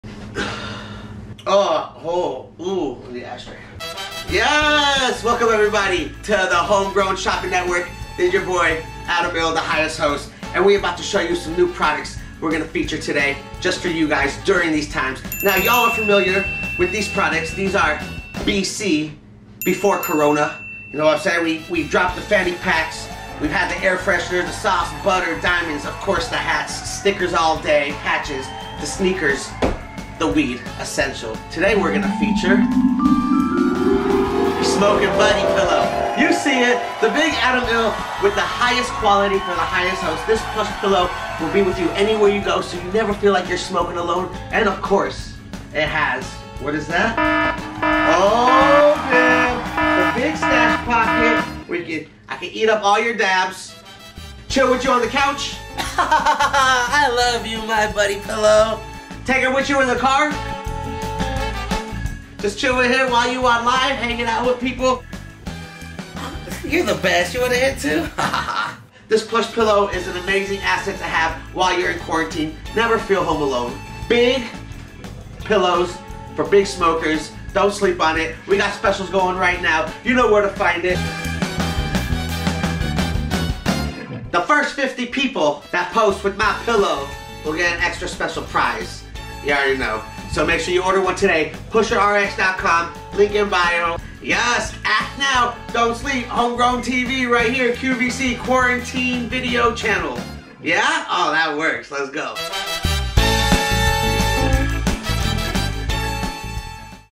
Quarantine Video Channel presents Adam Ill's "Smoking Buddy Pillow" with 420 Stash Pocket
Introducing the "Smoking Buddy pILLow."

'HSN' (Homegrown Shopping Network) and 'QVC' (Quarantine Video Channel) introduce the iLLsential collection.

This plush pILLow is made with the highest quality fabrics for the highest host.

Hang out and chill with Adam every time you sesh!

Don't know where to put your rosin, flower or vape?!? Adam's got you with his built in stash pocket.

Don't sleep and order one today.